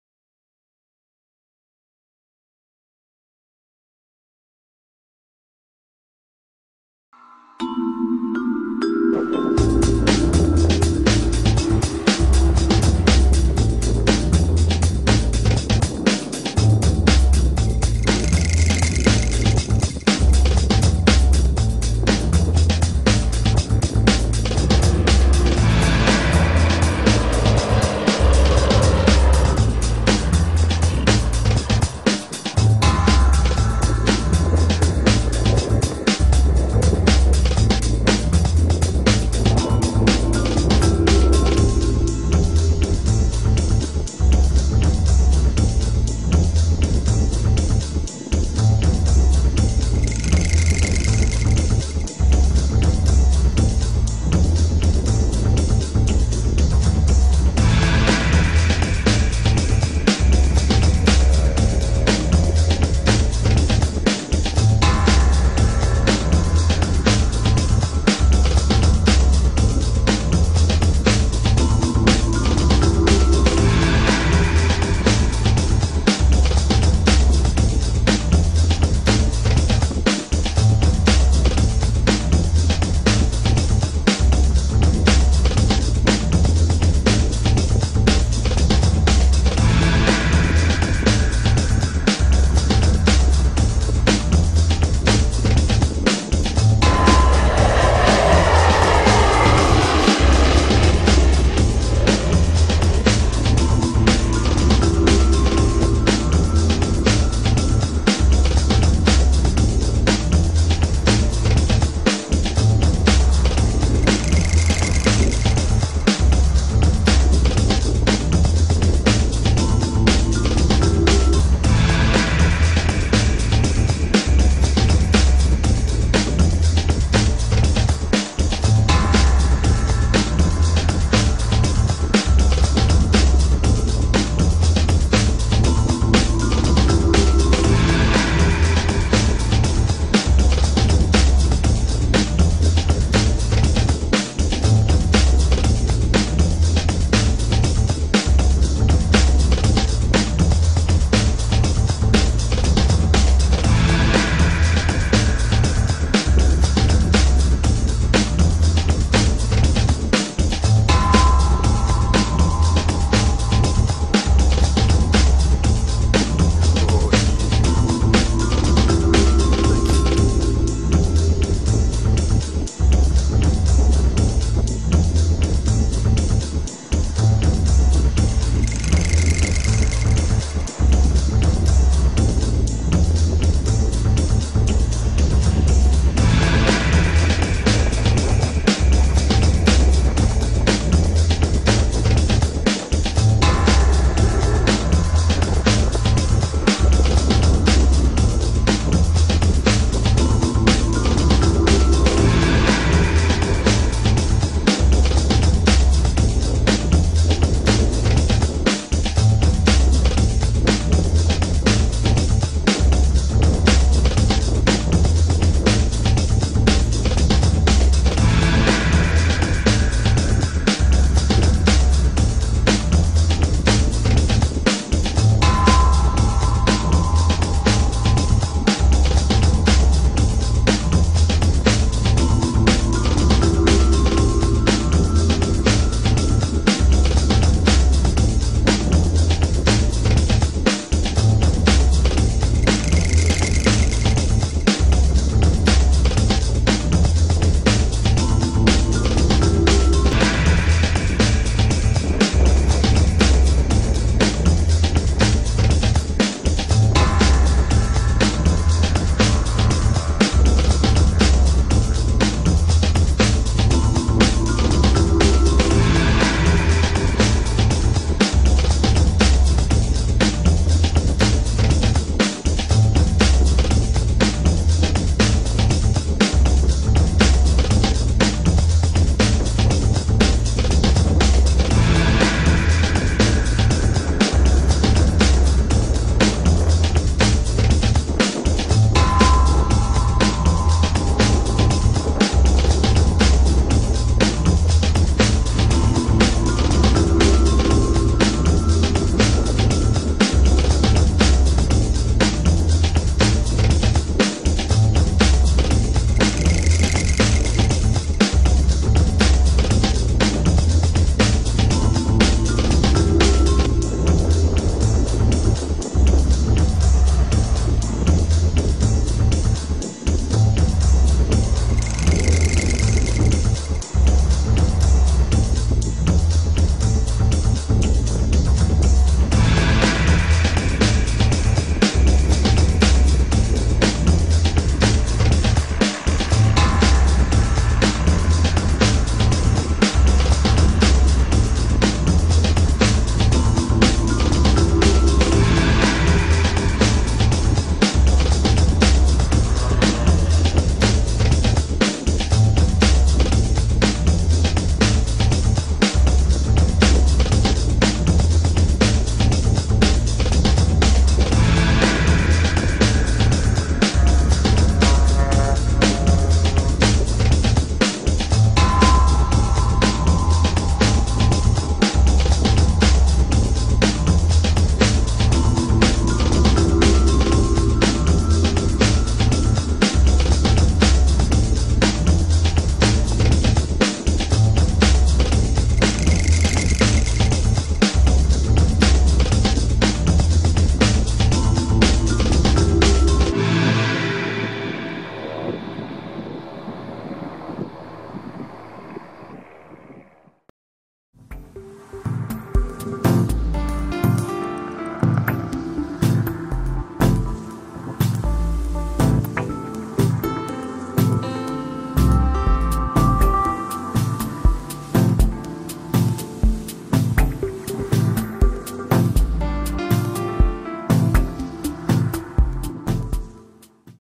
Aircraft Takeoffs and Landings at San Carlos California airport.
Takeoffs and landings with aircraft model listings.